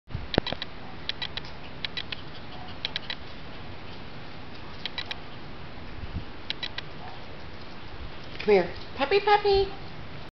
Sweet Thang - 6/30/11 - Age: 5 weeks tomorrow
His morning romp with Daisy Dog